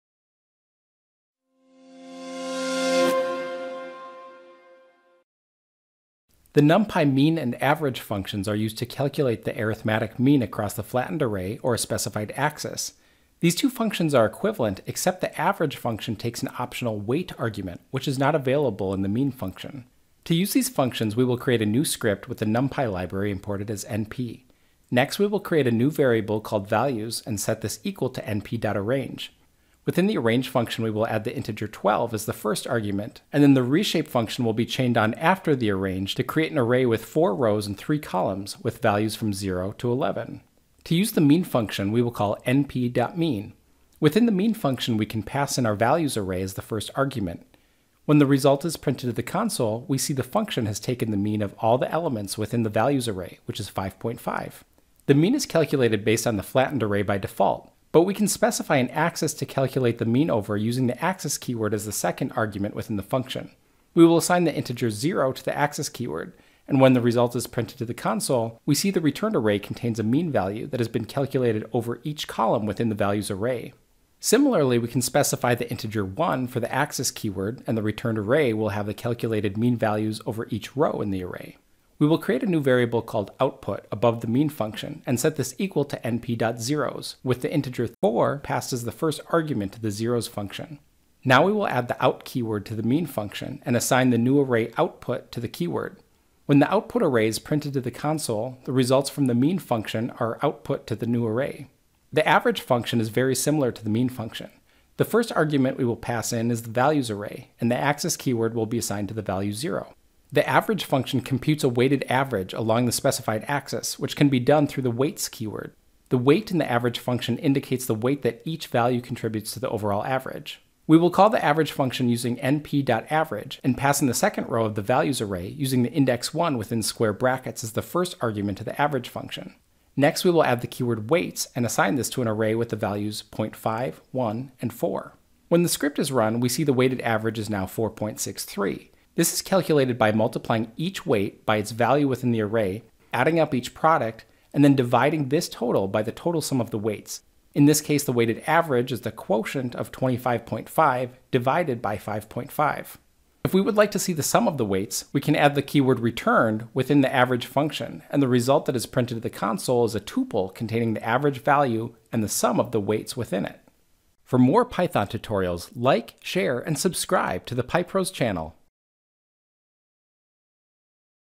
Python | NumPy Mean and Average
The NumPy mean and average functions are used to calculate the arithmetic mean across the flattened array or a specified axis. These two functions are equivalent except the average function takes an optional weight argument which is not available in the mean function.